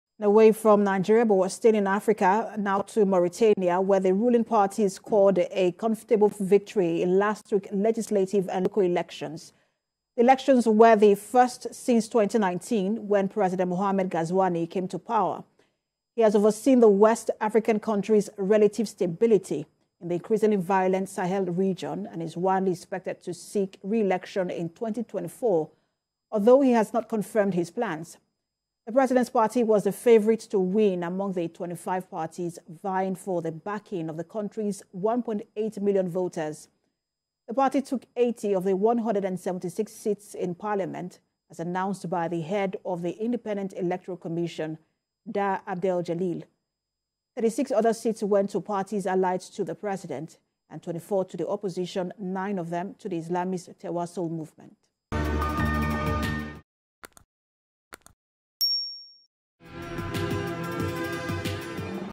Ruling Party Wins Majority Seats in Local and Legislative Polls
The ruling party scored a comfortable victory in last week's legislative and local elections.
The elections were the first since 2019, when President Mohamed Ghazouani came to power.
He has overseen the West African country's relative stability in the increasingly violent Sahel region and is widely expected to seek re-election in 2024, although he has not confirmed his plans.
The president’s party was the favourite to win among the 25 parties vying for the backing of the country's 1.8 million voters.
The party took 80 of the 176 seats in parliament as announced by the head of the independent electoral commission, Dah Abdel Jelil.